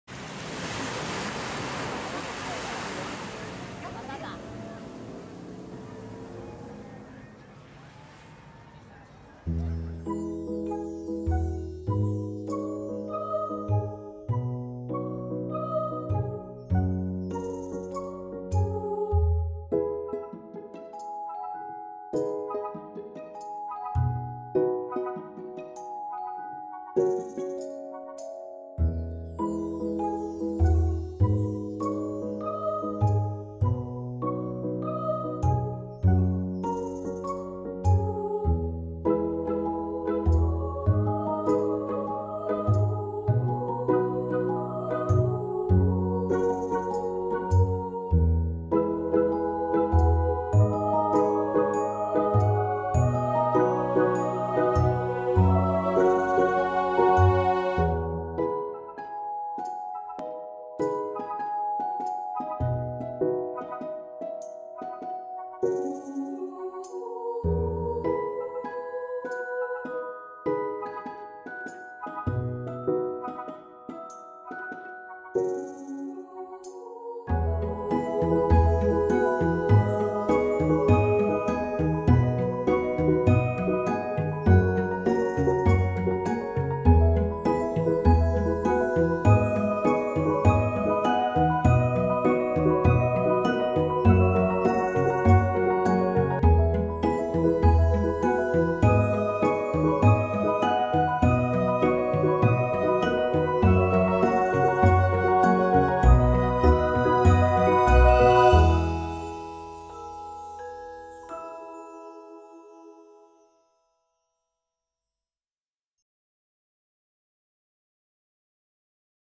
Donostiako Gabonetako Azoka 2018-19
Vídeo realizado para el mercado de Navidad de Donostia/San Sebastián.

Donostiako Gabonetako Azokaren laburpen bideoa.